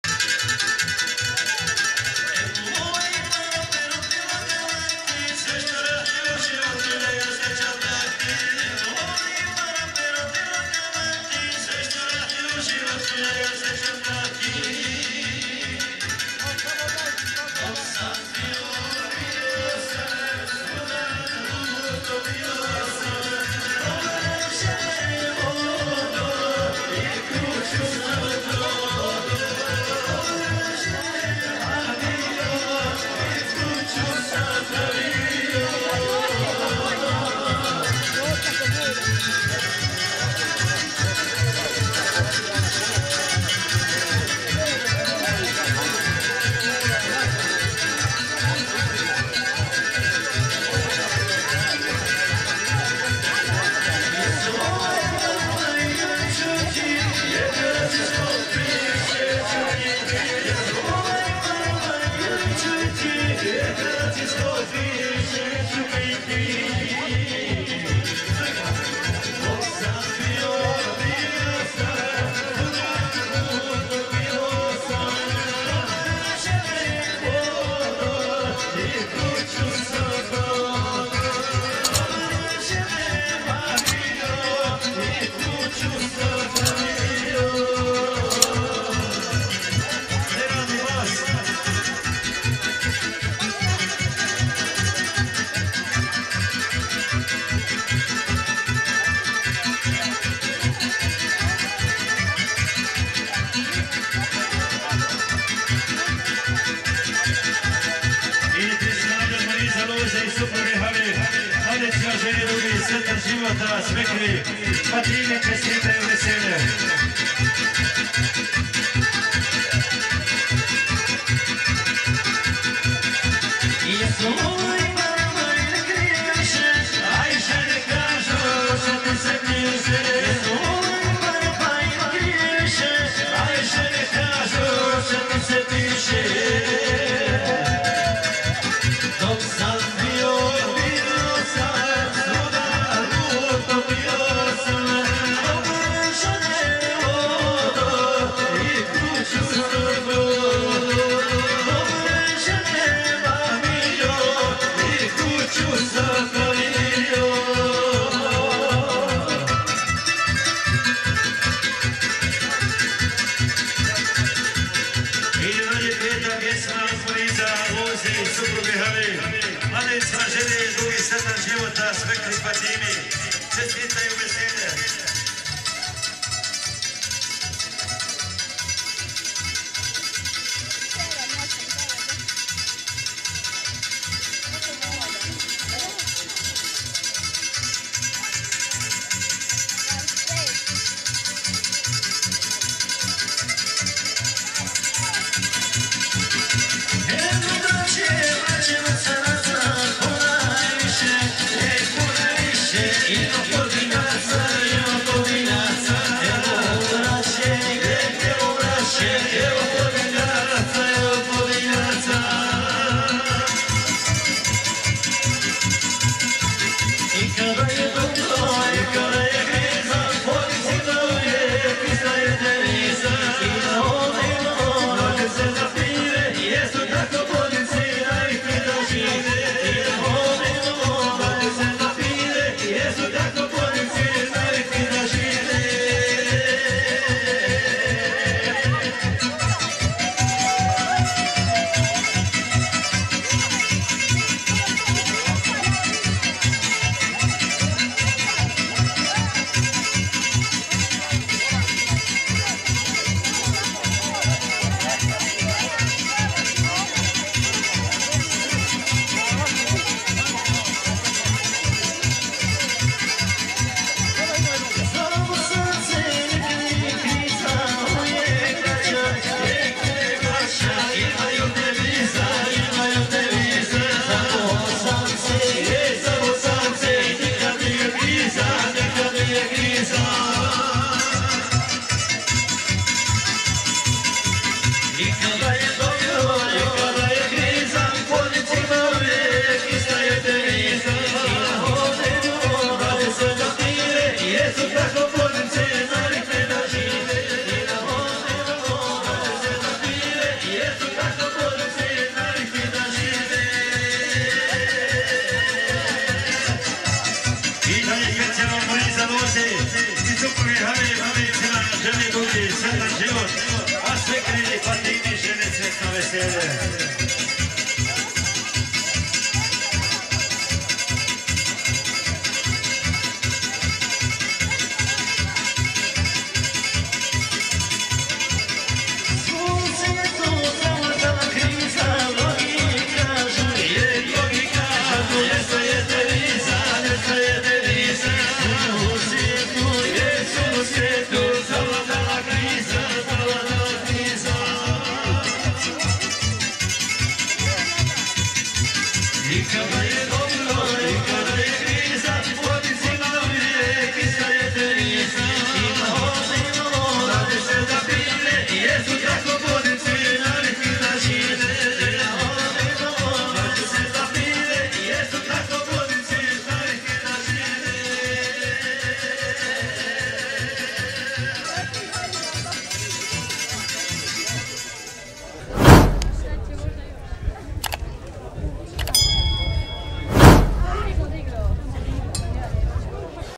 Kolo uz pjesmu Tinja Srebrenik. Muzika Edo & Sateliti Drine.
Kolo uz pjesmu sa svadbe: Saida Mustafića & Erne Nukić. Tinja Srebrenik: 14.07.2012. Snimatelj: Ekrem Brigić.